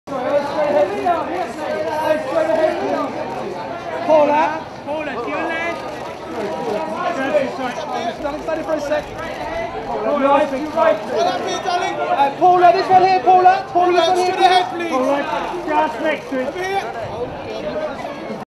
Paula Lane at 'Olympus Has Fallen' UK Premiere at BFI IMA...
Paula Lane at 'Olympus Has Fallen' UK Premiere at BFI IMAX on April 03, 2013 in London, England